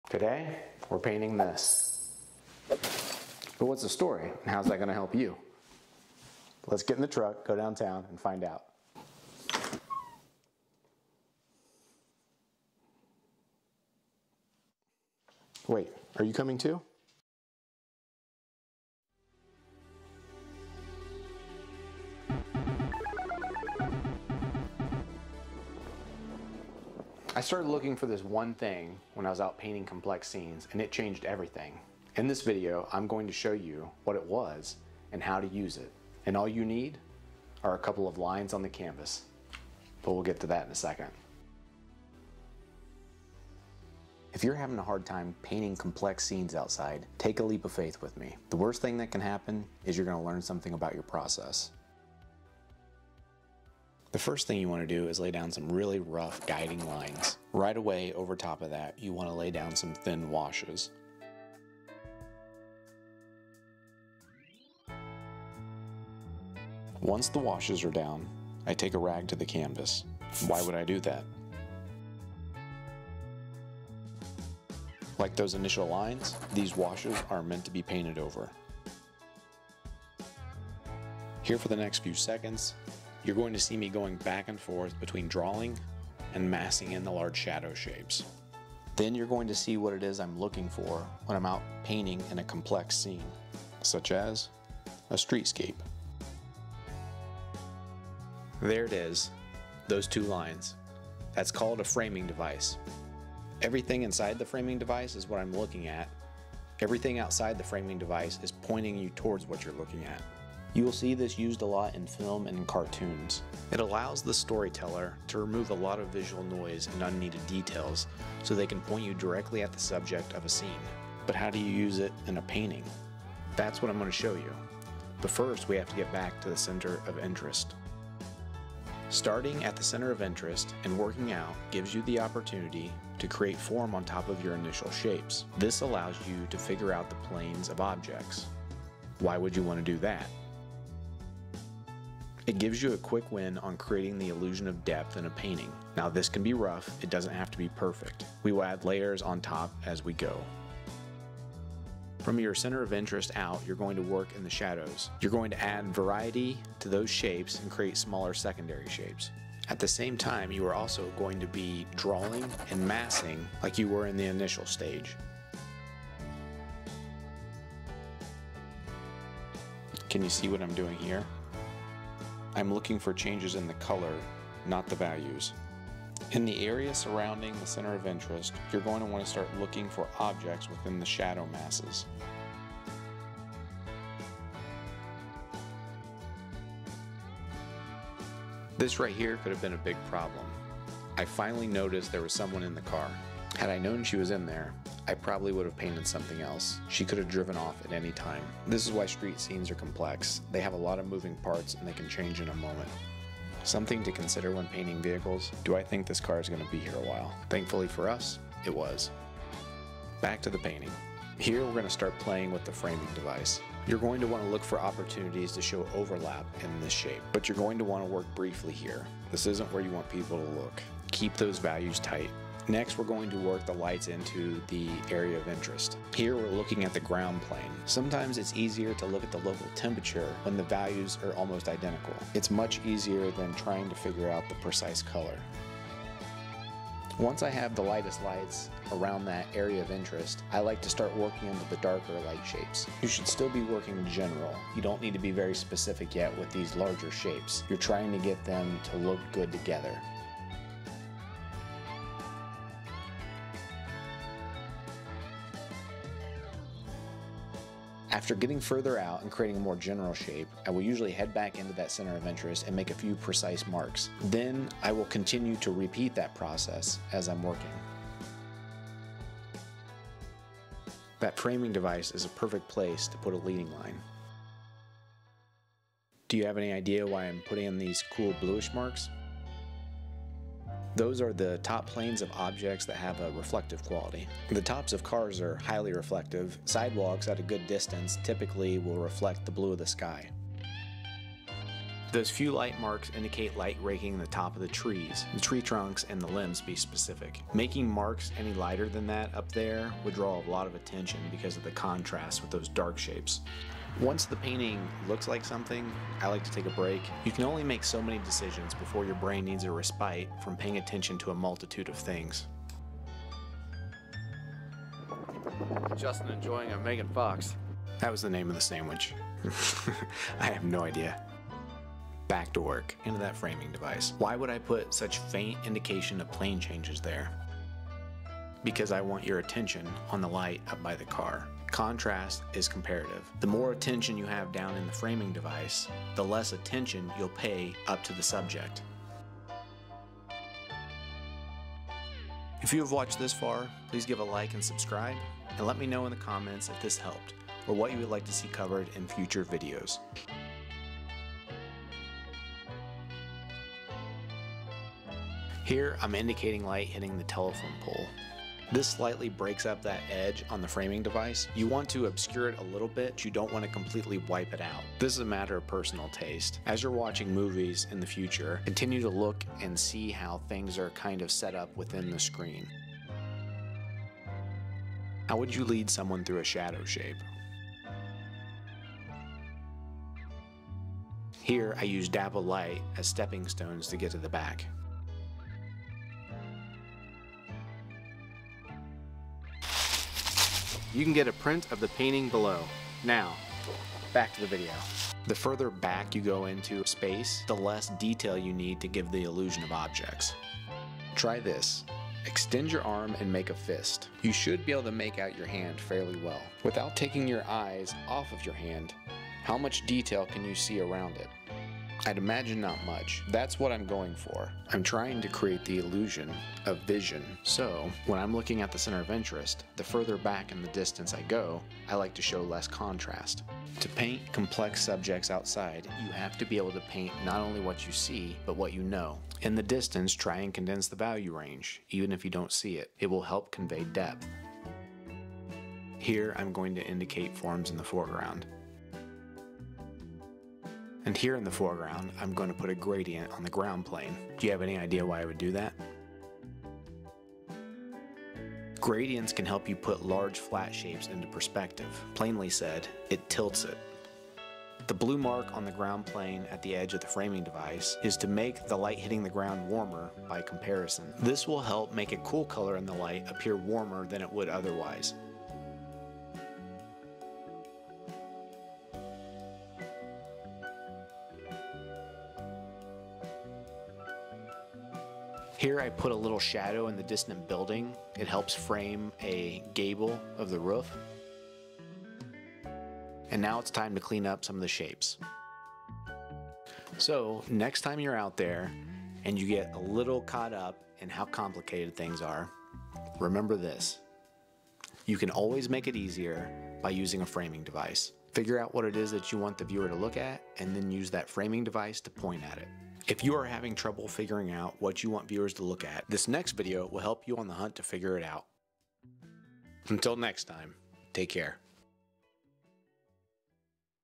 Unlocking the Secret of Painting Complex Landscapes Fast
Helping you paint your next complex scene outside. Bring the beauty into your home! Giclee Prints Available